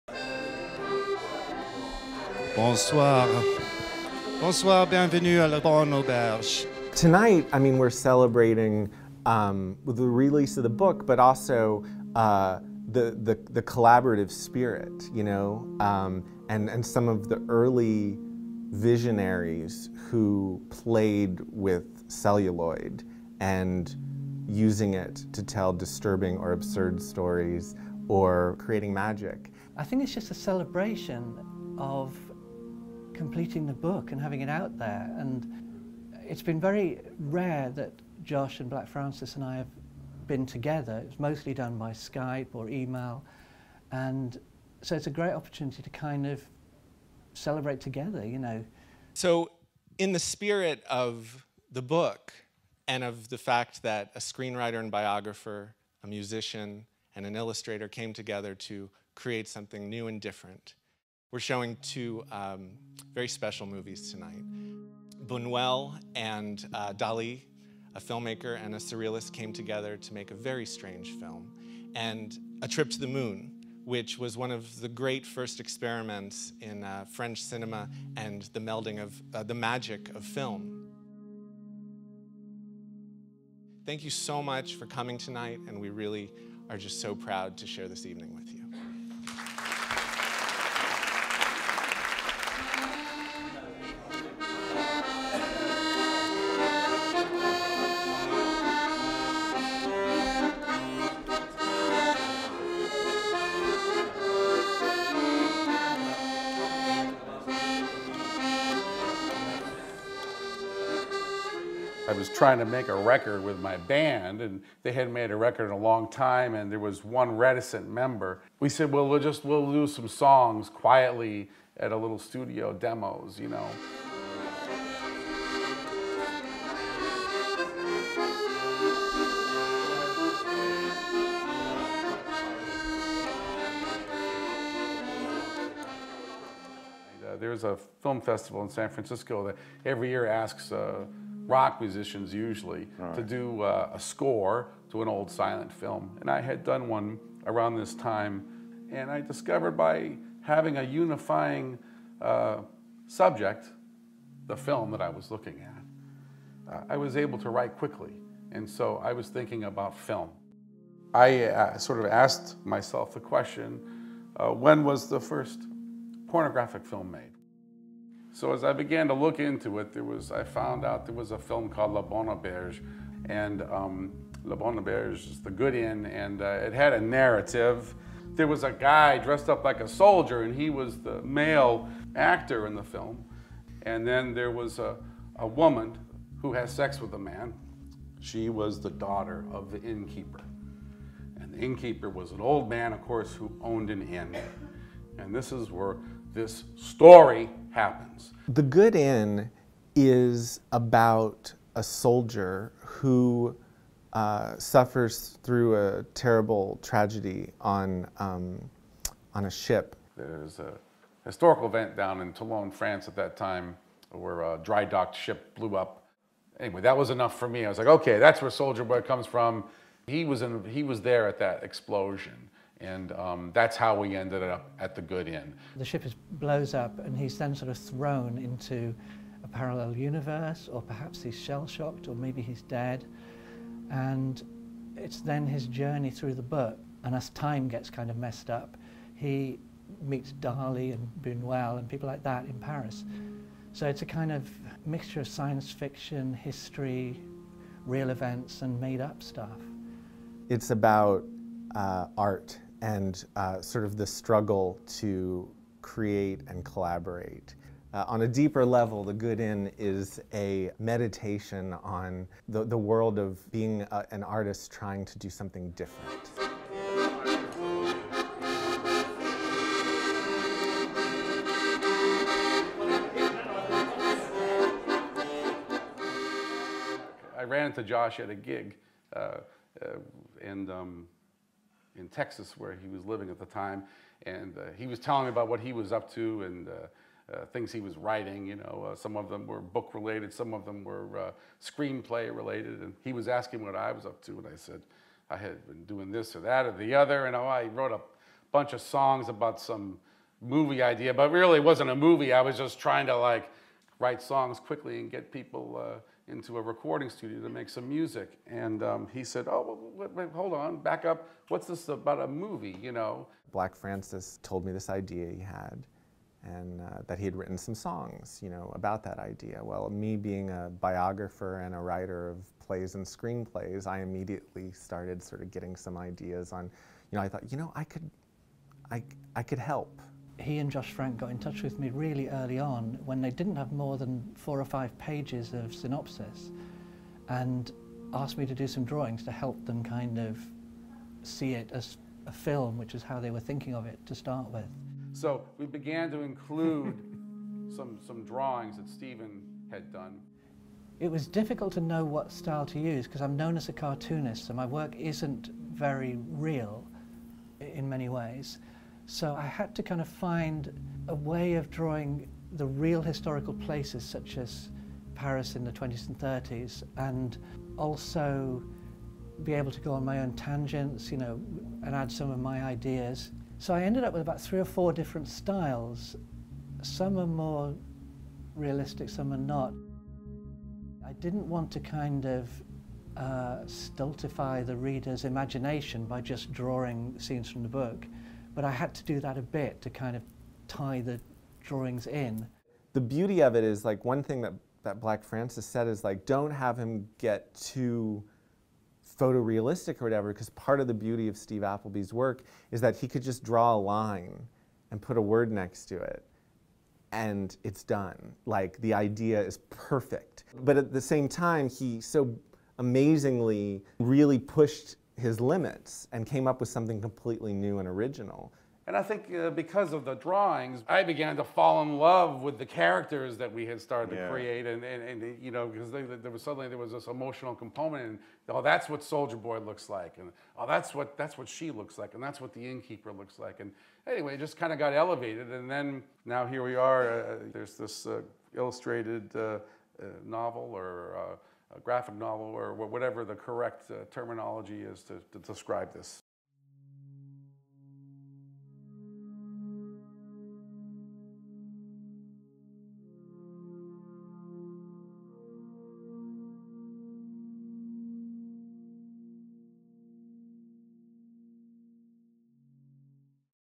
A Night At The Good Inn, with Black Francis, Josh Frank & Steven Appleby
To celebrate the UK release of The Good Inn, Pixies frontman Black Francis joins co-author Josh Frank and artist Steven Appleby for an evening of film and literature at The British Library in London.

Here they discuss the genesis of the project and the pioneers of early French cinema who inspired it.

Published by SelfMadeHero, The Good Inn is a bold and visually arresting illustrated novel about art, conflict and the origins of cinema.